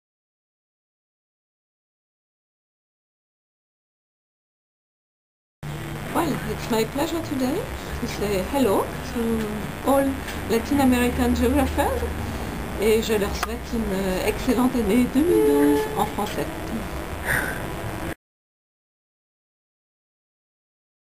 Saludo de la Dra. Denise Pumain
Saludo de la geógrafa Denise Pumain a los geógrafos latinoamericanos durante su corta estadía en Lima, Perú en diciembre del 2011. La doctora Pumain es experta en Modelización y sistemas urbanos. Ha recibido el premio Vautrin Lud el año 2010.